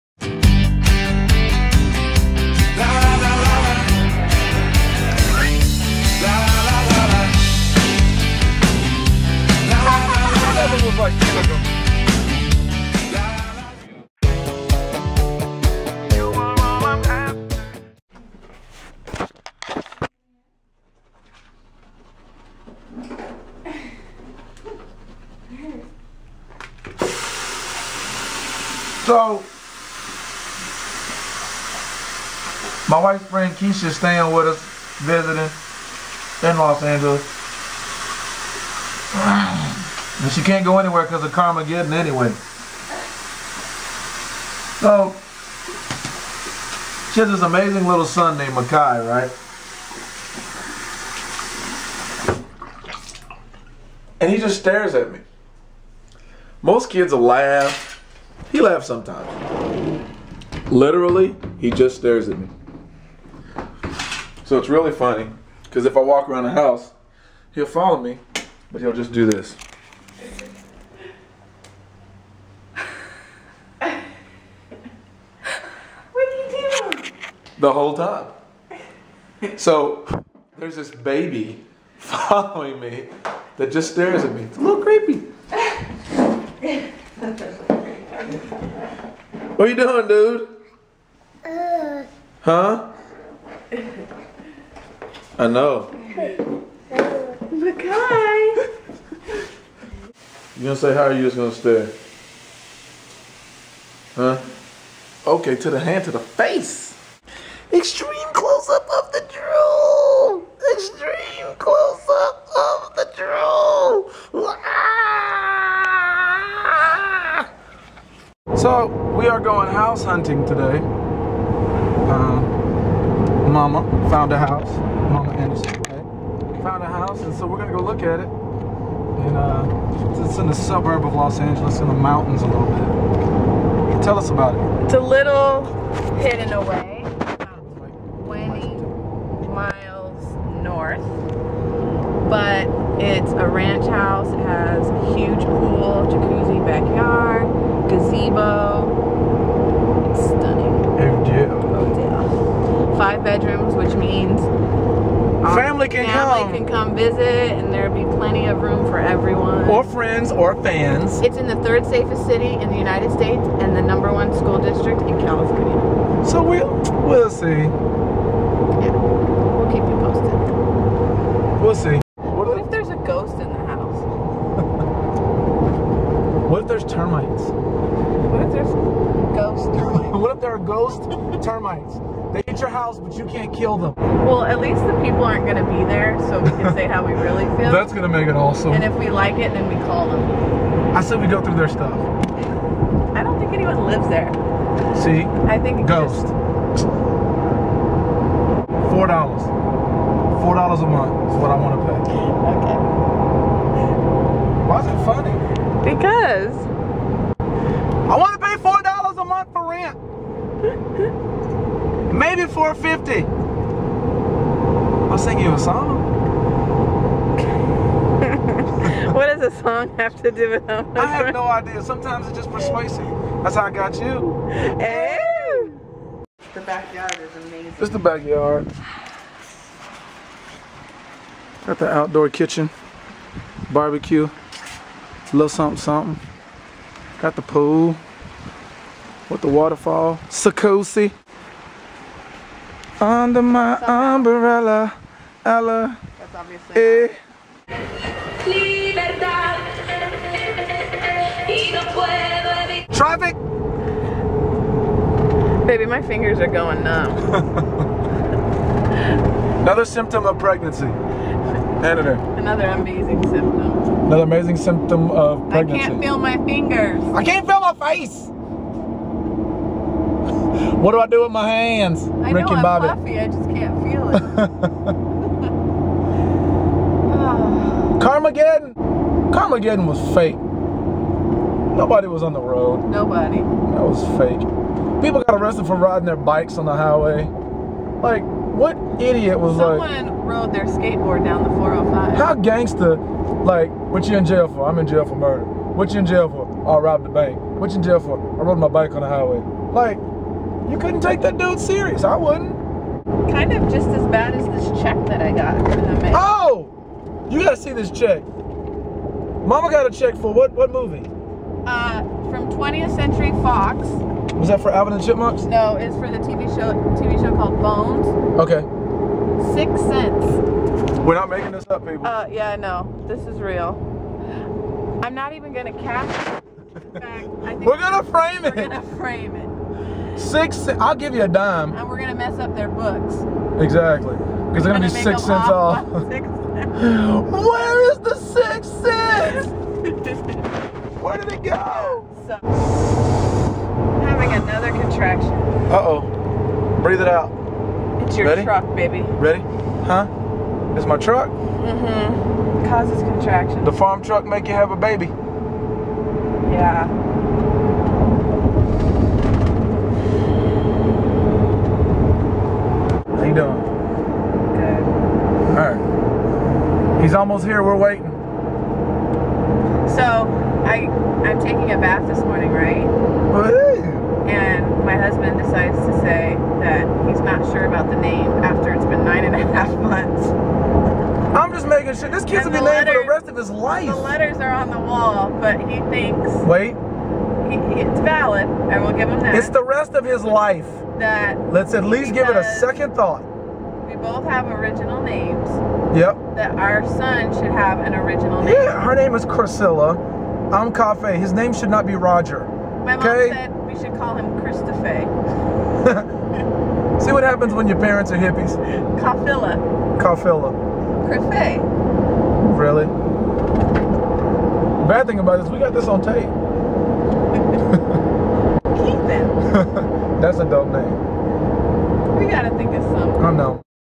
Baby Fever
Me and Criscilla had a friend staying the night and her new baby. Then we take a trip to check out a house. Always an adventure.